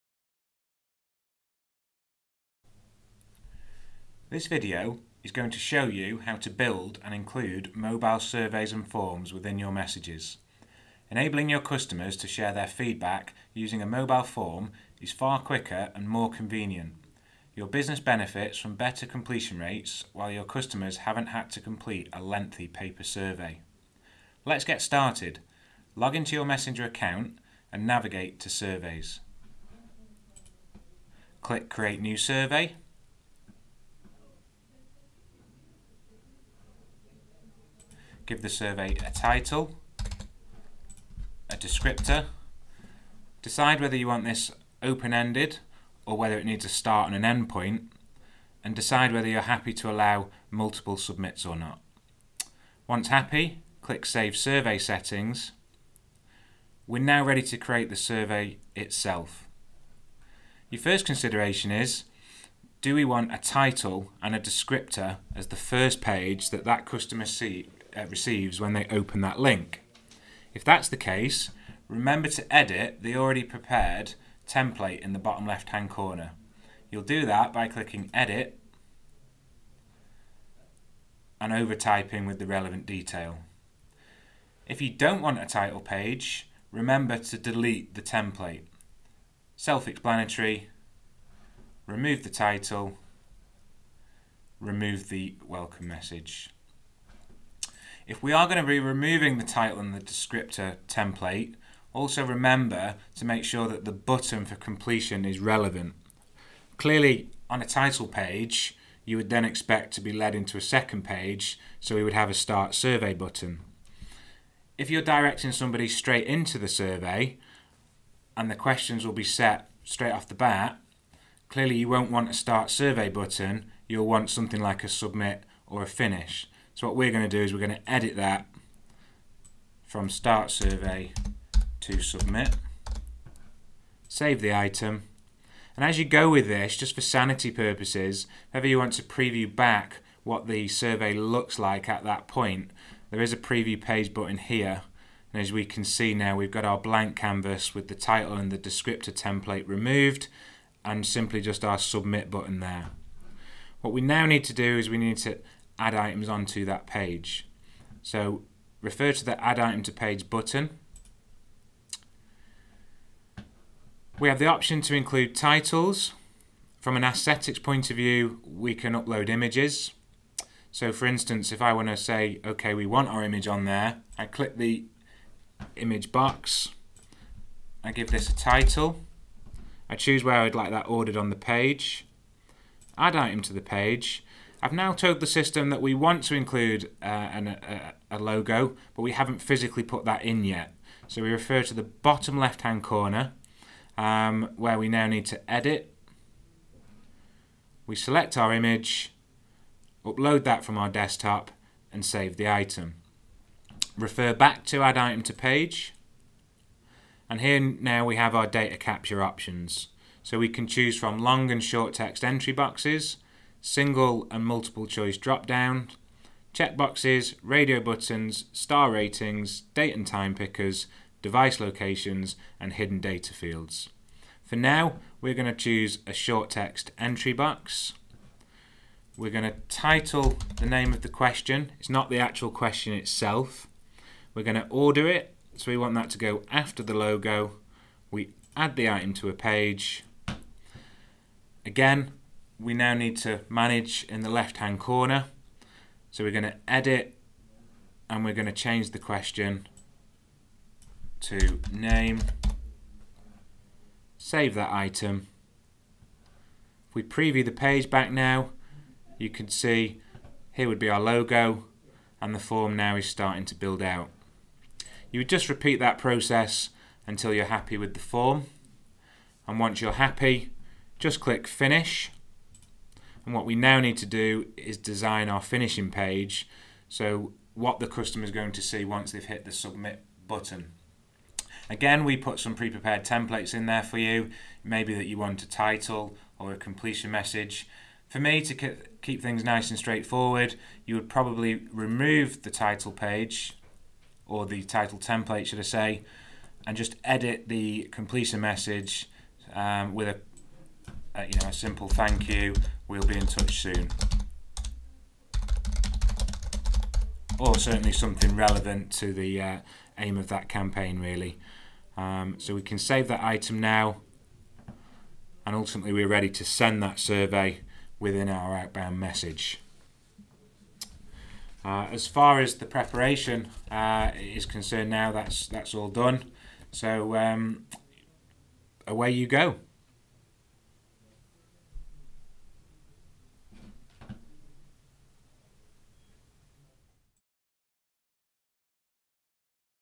How to create SMS surveys | Textlocal
In this tutorial, we will walk you through how to create an SMS survey to use in your campaigns in your Textlocal Messenger account.